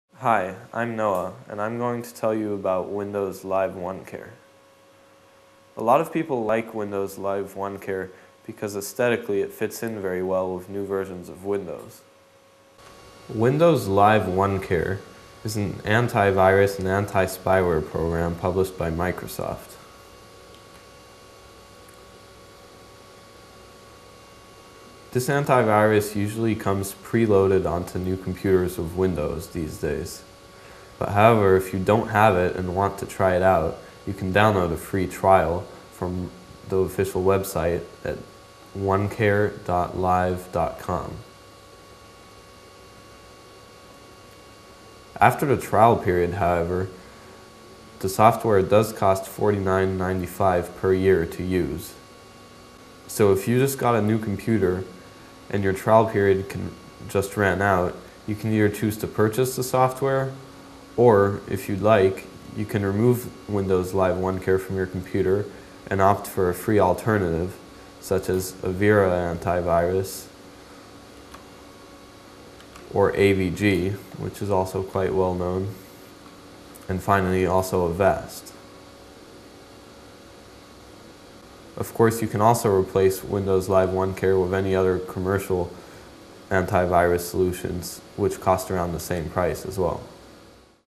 Computer Files & Tips : About Windows Live OneCare
Windows Live OneCare is an anti-virus and anti-spyware program published by Microsoft that can be downloaded at onecare.live.com. Find out how computers often come with trial versions of Windows Live OneCare and how much the software costs with help from a Web designer in this free video on computer help and tech support.